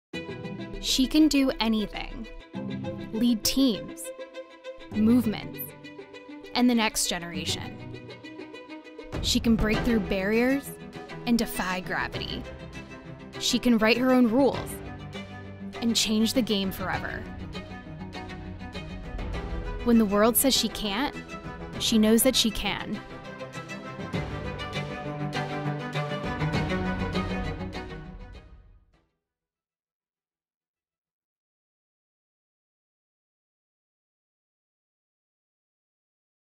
YES SHE CAN | Microsoft In Culture
This InternationalWomensDay, we’re celebrating the trailblazers who inspire all of us to break down the barriers that get in the way. We stand behind all girls and women who rise above the voices saying, “you can’t.” Because we, and millions of others, know you can.